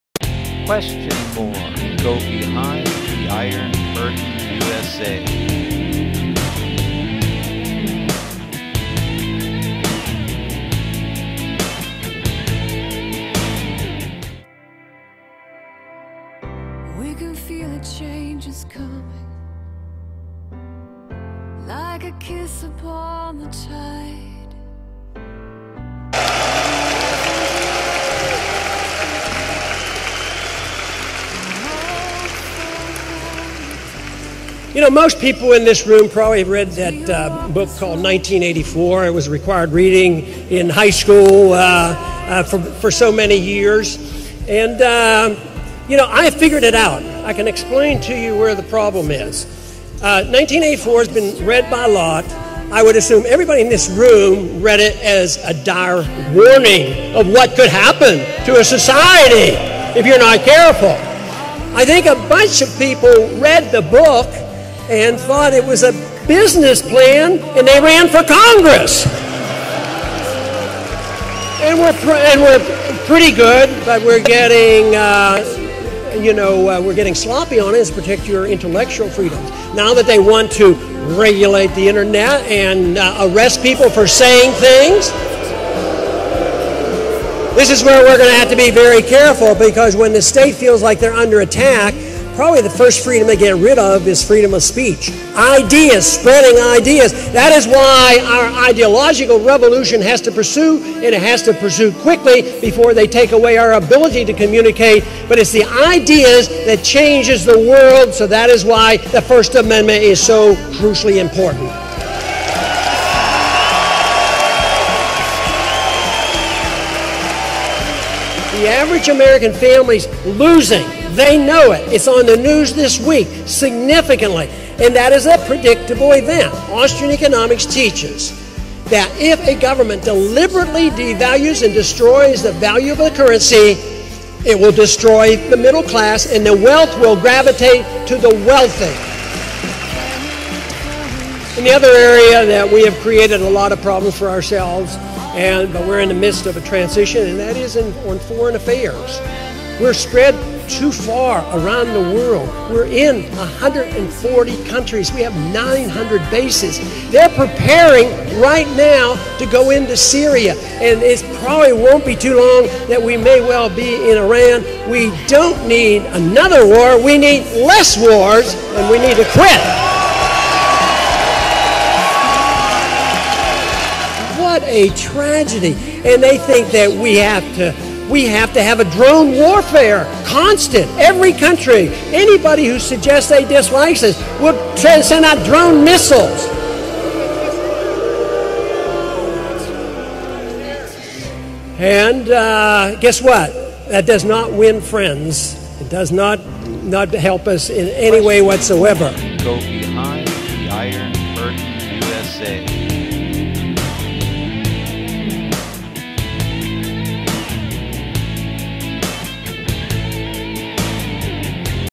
Republican National Convention did want Ron Paul to talk about Drones and Loss of Liberty
Loyal Ron Paul supporters from gathering at what is being called Ron Paul Festival or PaulFest . Listed to Ron Paul  with Gary Johnson on War and Iran,Afghanistan and Pakistan. Americas two party system is a failure is the consensuses of most of the followers. Ron Paul has won without being elected says one of the followers. He has won the hearts and minds of more people that any recent candidate in history,  without being elected. From years to come people will adopted and embrace the ideas and issues he has made know.